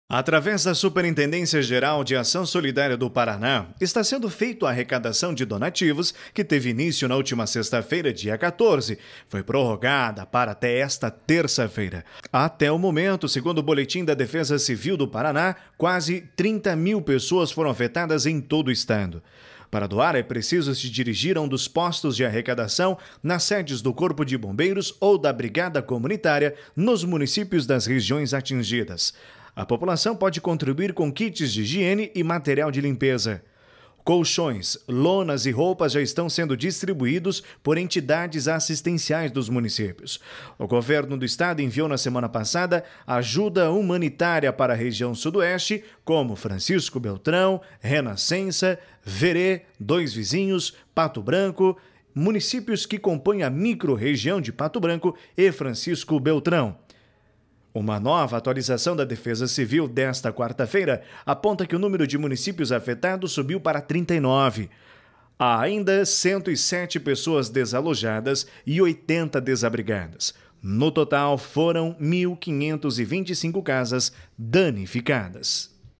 Campanha que arrecada donativos para famílias atingidas pelas chuvas foi prorrogado
Ações e Campanhas: Campanha que visa arrecadação de donativos para famílias atingidas pelas chuvas deste mês seguem até esta próxima terça-feira (25).